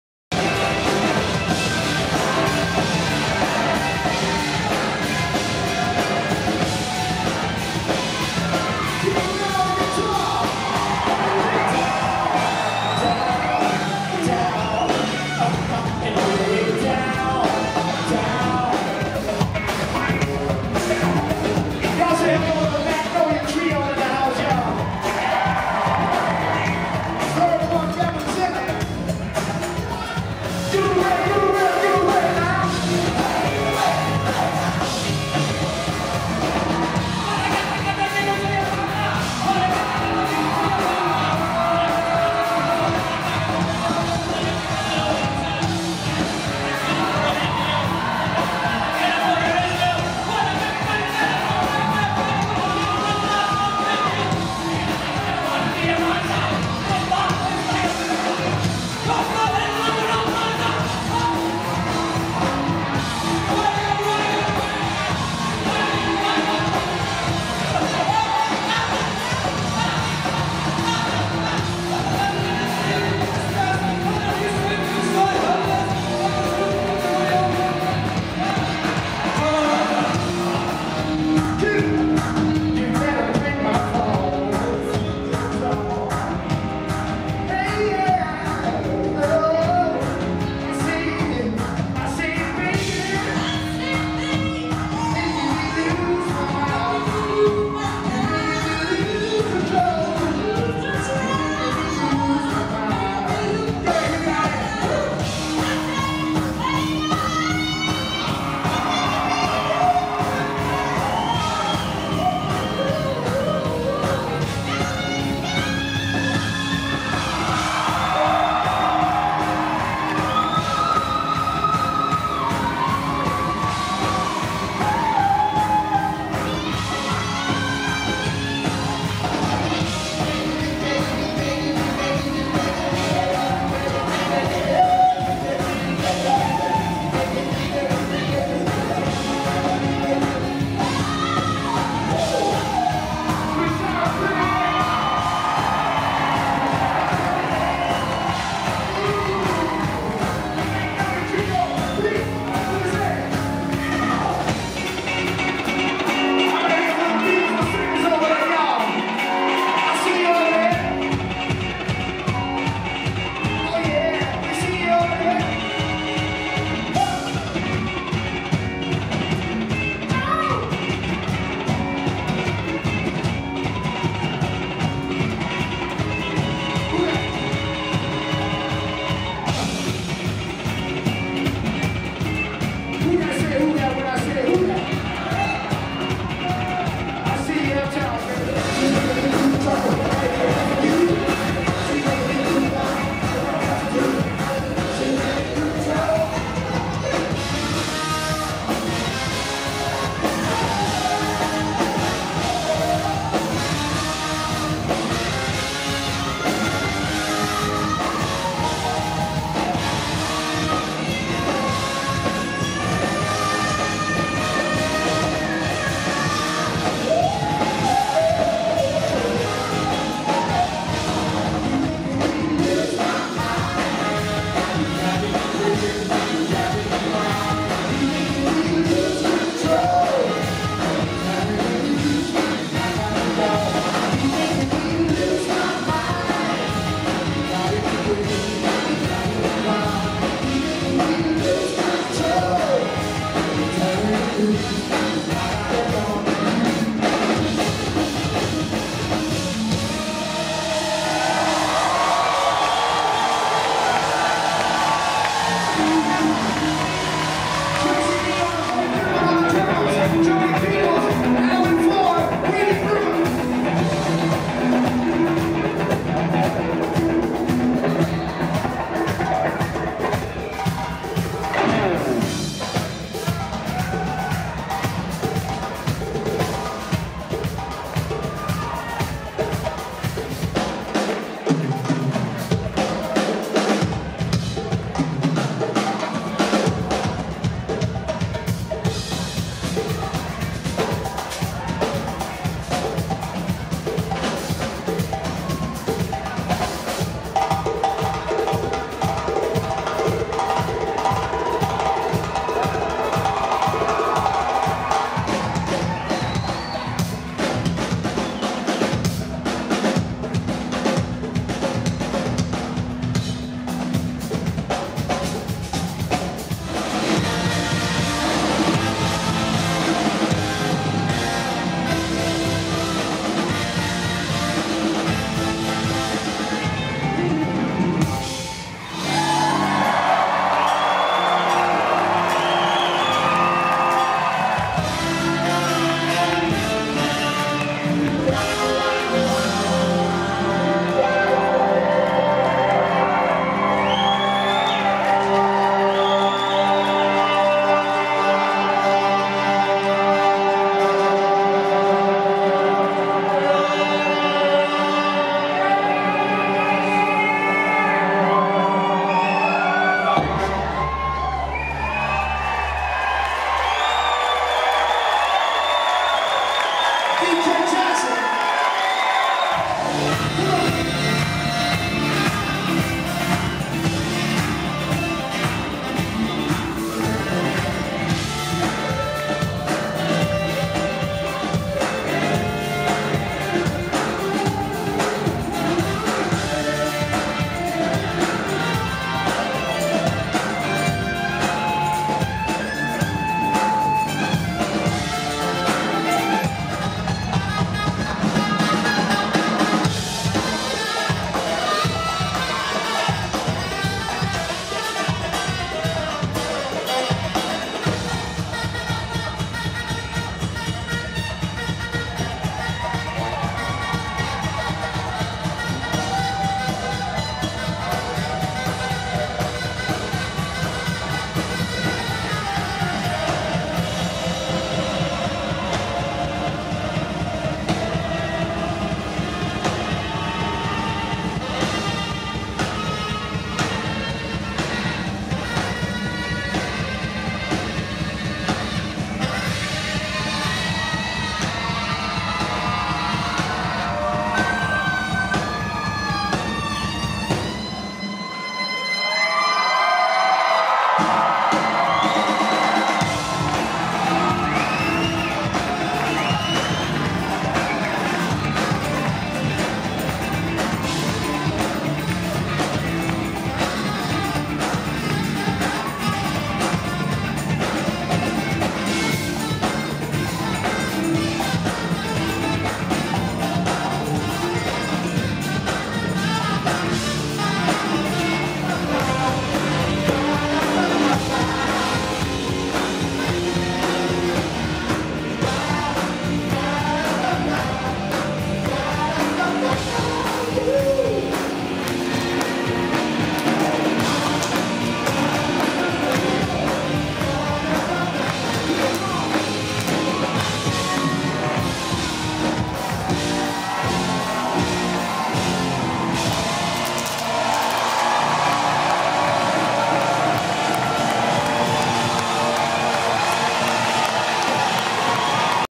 Trombone Shorty & Orleans Avenue at The Wilma 8.14.17
A variety of video clips from Trombone Shorty & Orleans Avenue at The Wilma, Missoula MT. - 8.14.17